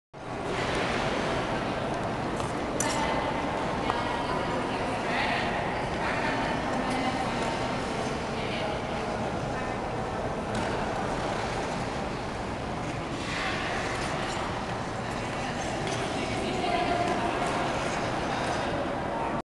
1-17-08 - 2 Handed Death Spiral
Working on improving the forward inside death spiral...